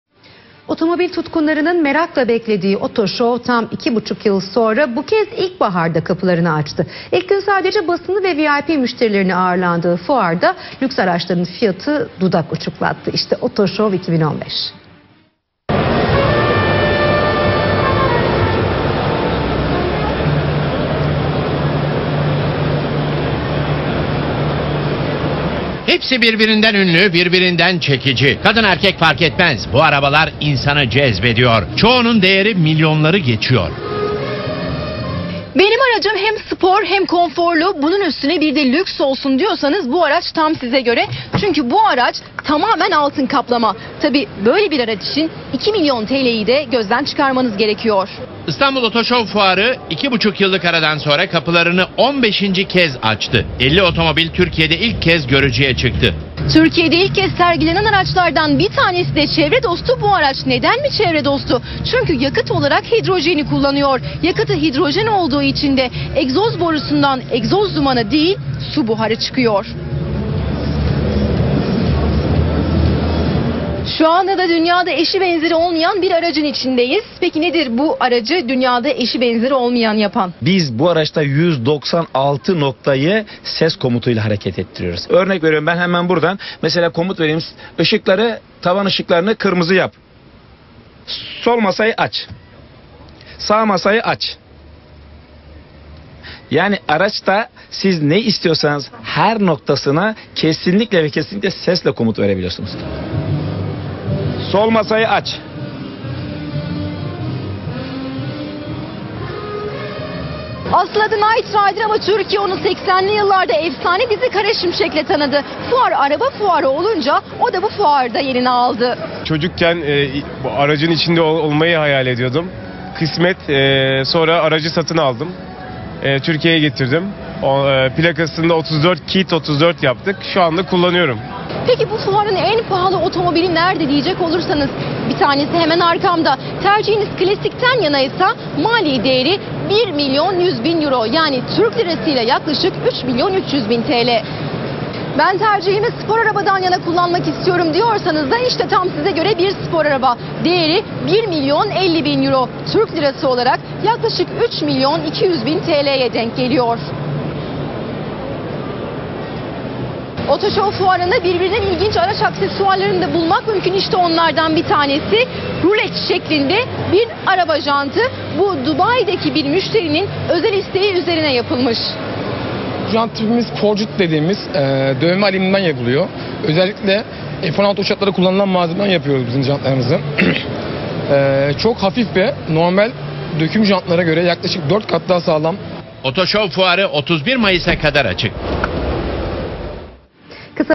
21.05.2015 GYRWHEELS-AUTOSHOW 2015
GYRWHEELS  Genel Koordinatörü Recep Küçükhüseyin ürünlerini anlatıyor.